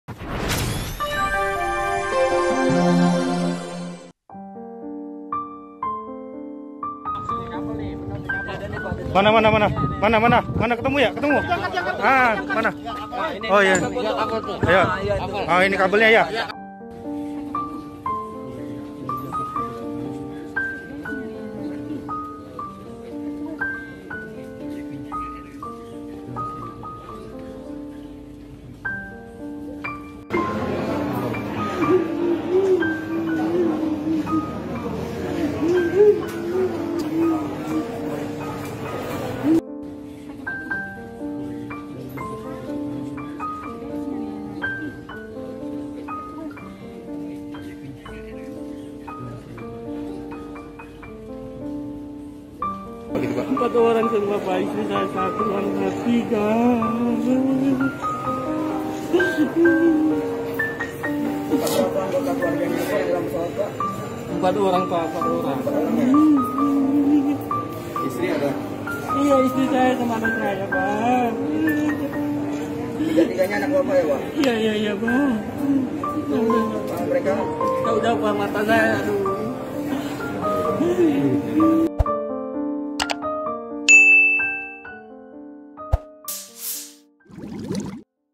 Isak Tangis Keluarga Penumpang Pesawat SJ 182
Pesawat Sriwijaya Air  dengan kode penerbangan SJ-182 dikabarkan jatuh di Kepulauan Seribu, Sabtu (9/1/2021). Pesawat tersebut membawa penumpang sekitar 62 orang.

Salah satunya, seorang suami bernama Yaman menanti kabar istri dan ketiga anaknya di Bandara Supadio, Pontianak.

Terlihat juga keluarga penumpang SJ-182 menyambangi Bandara Supadio, Pontianak menunggu kepastian dari maskapai.

Berdasarkan informasi dari Basarnas, pesawat itu hilang kontak di sekitar Pulau Laki dan Pulau Lancang, Kepulauan Seribu.